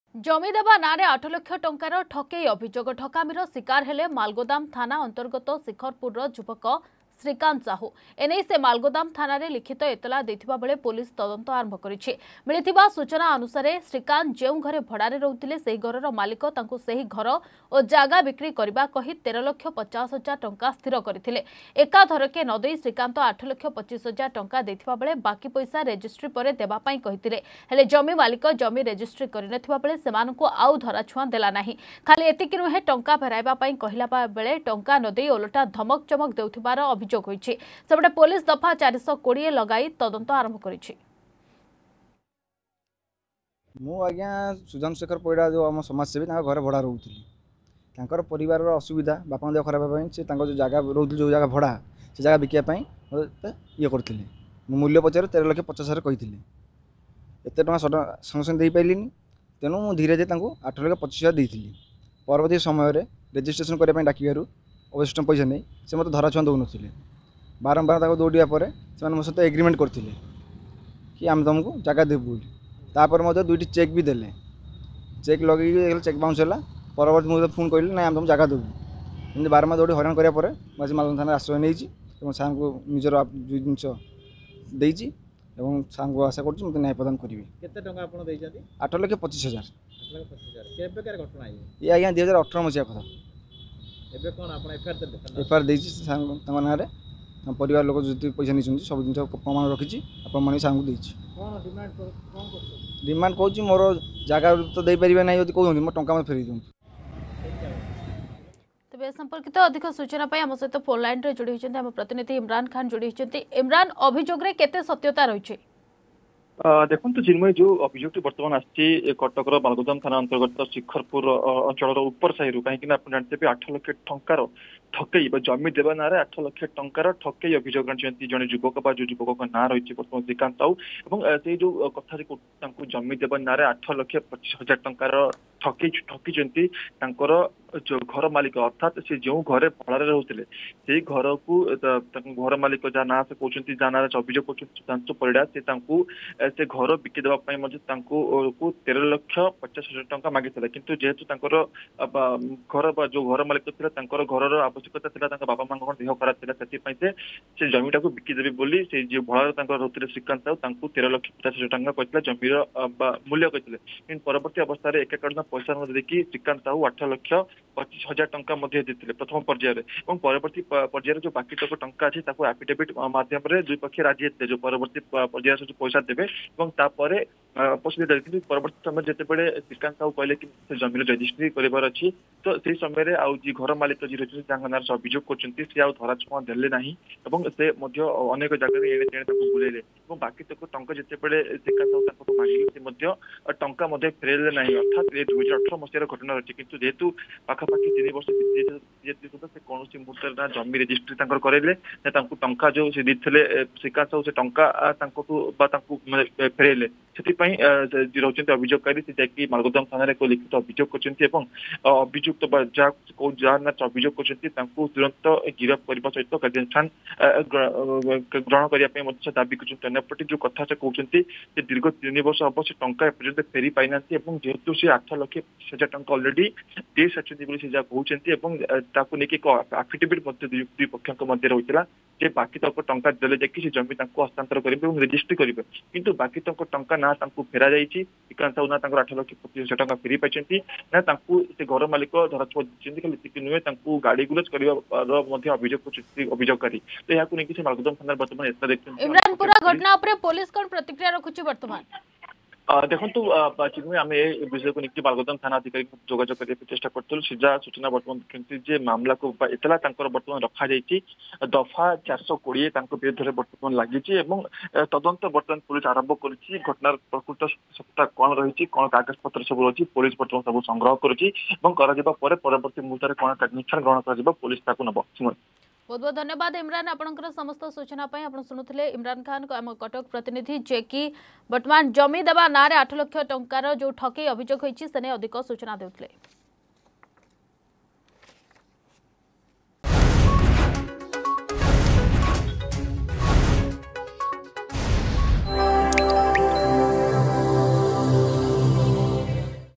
ଜମି ଦେବା ନାଁରେ 8 ଲକ୍ଷ ଟଙ୍କାର ଠକେଇ ଅଭିଯୋଗ...
ଜମି ଦେବା ନାଁରେ 8 ଲକ୍ଷ ଟଙ୍କାର ଠକେଇ ଅଭିଯୋଗ... ମାଲଗୋଦାମ ଥାନାରେ ଲିଖିତ ଅଭିଯୋଗ କଲେ ଶିଖରପୁରର ଯୁବ ବ୍ୟବସାୟୀ ଶ୍ରୀକାନ୍ତ ସାହୁ... ଟଙ୍କା ମାଗିଲେ ମାଲିକ ଦେଉଛି ଧମକ. ପୋଲିସ ତଦନ୍ତ ଜାରି...